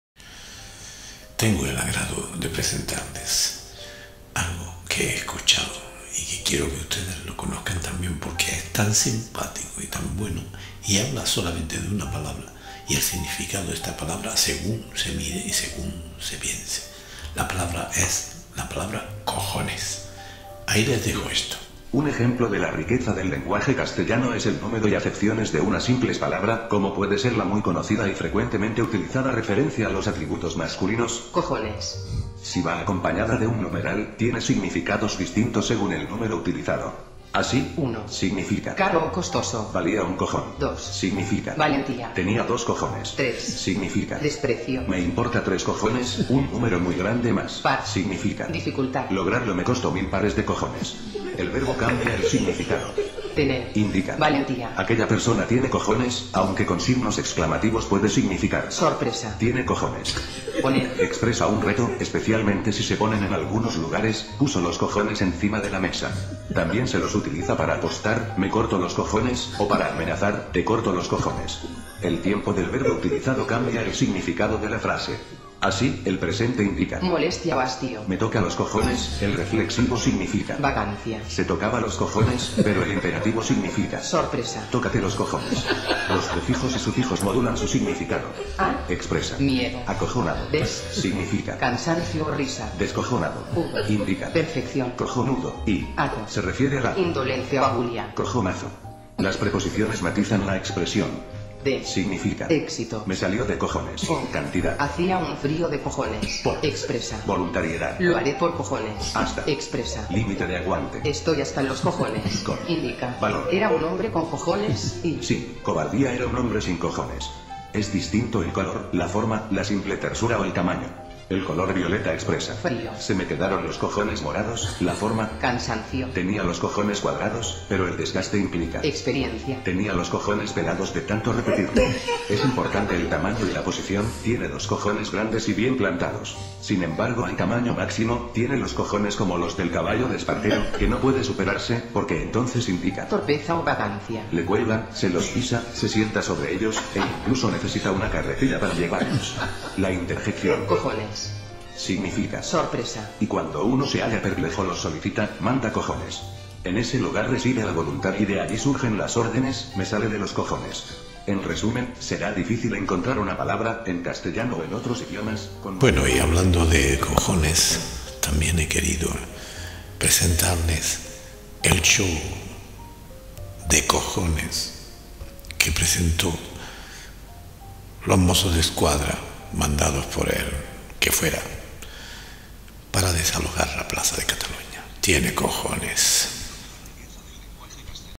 Manda Cojones
Como todo no va a ser cantar, aqui les dejo este video para que se lo pasen bien y se rian un rato, al mismo tiempo que ven como los Mozos de Escuadra de Barcelona se comportaron con los ciudadanos que se manifestaban en paz.